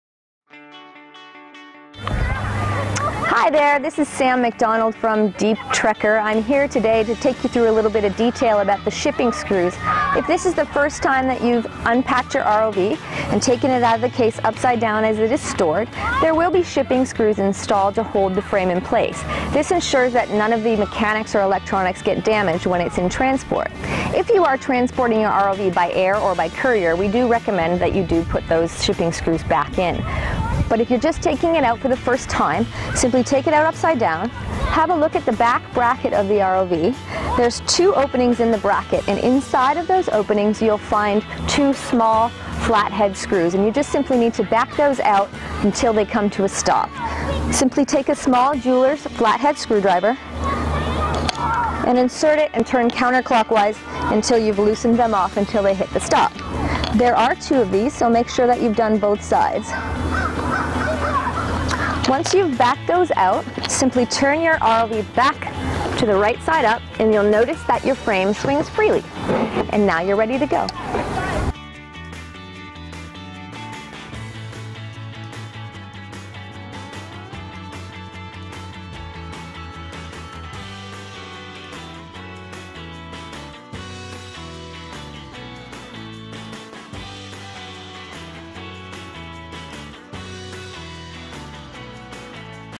How to Remove the Shipping Screws on the DTG2 | Deep Trekker
A step-by-step guide to removing the shipping screws on your DTG2. When you first receive your DTG2 underwater drone or ROV from the factory, shipping screws will be engaged to ensure there is no damage during transit. Before putting your ROV in the water, make sure you disengage the shipping screws.

0:00 Intro
0:10 Removing the Shipping Screws
1:11 Outro

Are you having trouble setting up your DTG2 ROV?